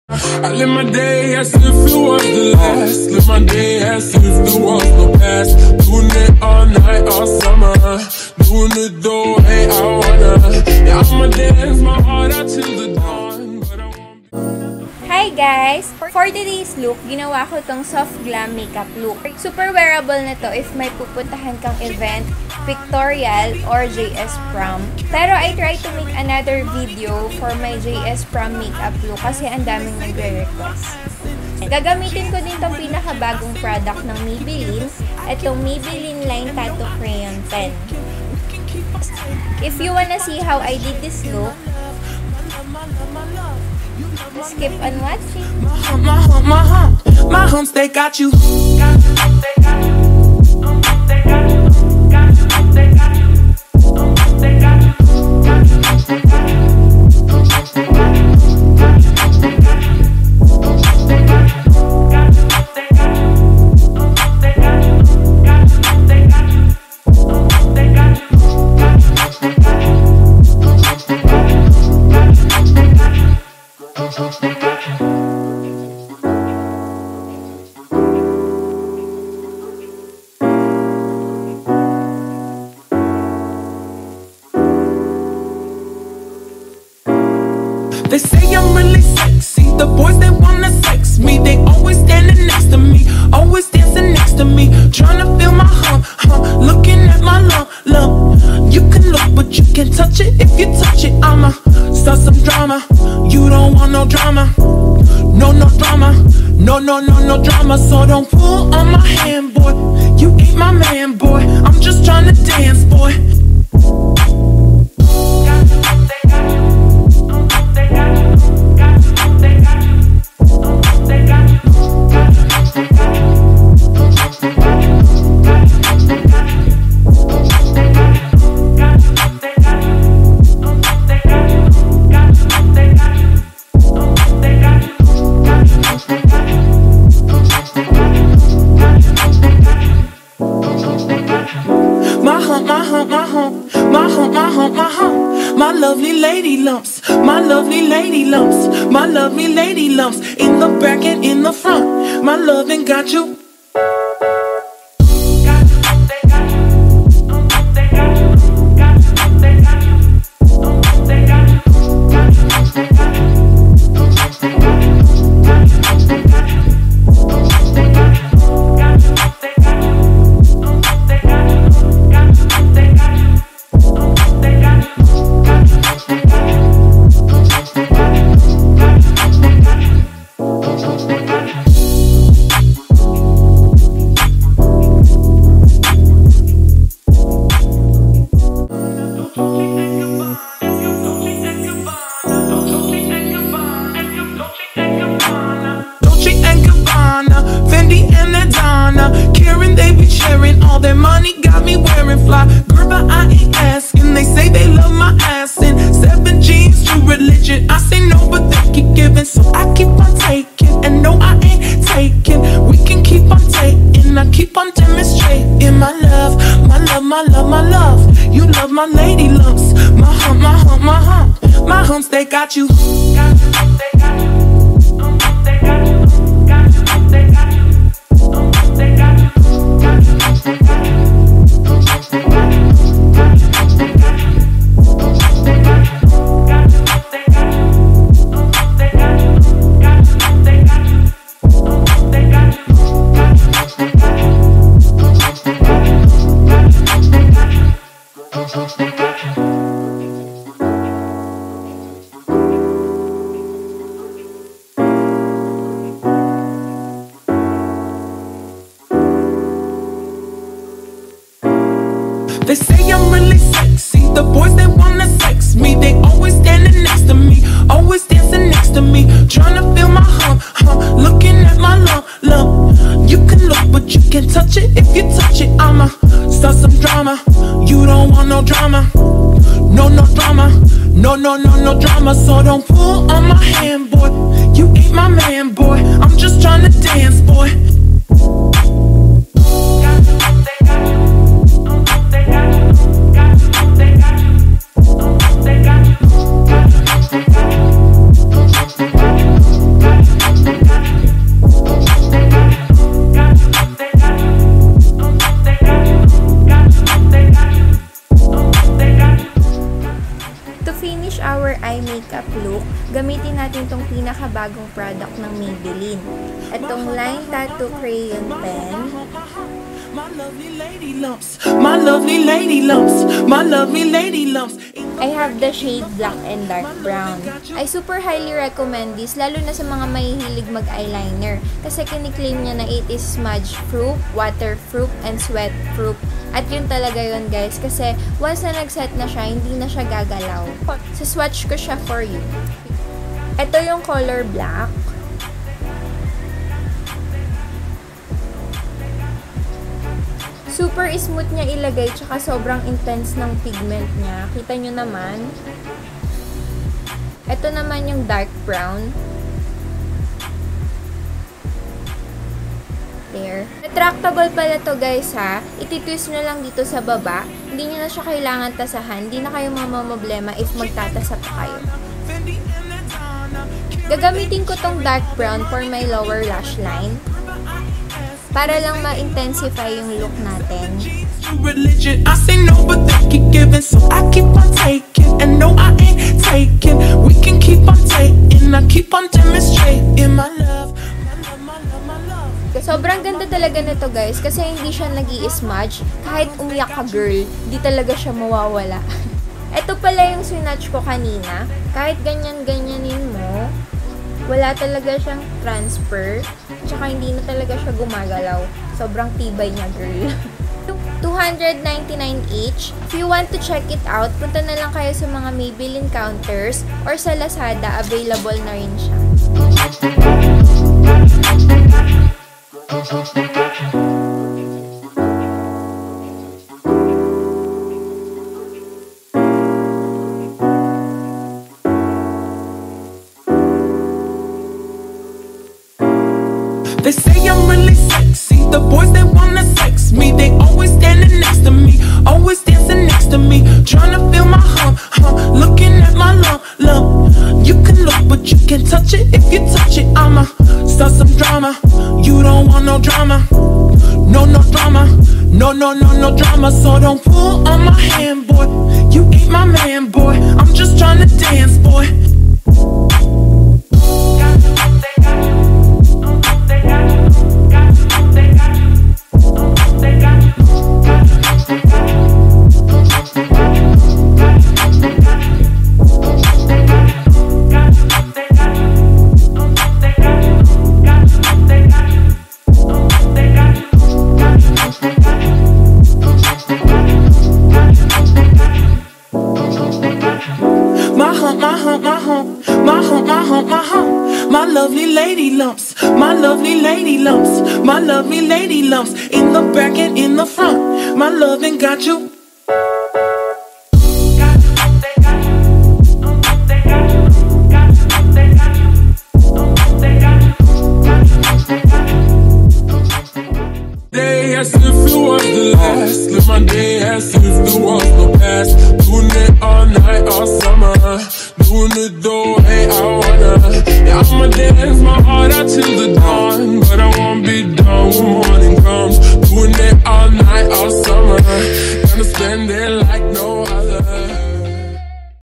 SOFT GLAM MAKEUP LOOK ft. Maybelline Line Tattoo Crayon Pen | Kaye Sierra (philippines)
xoxo, kaye ♡

Maybelline Line Tattoo Crayon Pen 299php each ( on sale for the whole month of february 249php only! )
shade • Dark Brown & Black
Available in all Maybelline Counters and in Lazada.

product(s) used :
-etude house fix and fix primer
-nivea lip balm
-maybelline fit me foundation (shade:120 classic ivory)
-maybelline instant age rewind concealer (shade: light)
-nichido loose setting powder (shade: creamy glow)
-impressed you palette by beauty creations
-i think i love you palette by colourpop
-magnum volume shot mascara by maybelline
-rimmel bronzer
-careline blush shade peachy glow
-wet n’ wild highlighter (shade: precious petals)
-ofra liquid lipstick shade manila and bys lip pencil shade flesh
-elf makeup mist and set
-lauv false eyelash

i’m not a pro, i just ♡ make-up!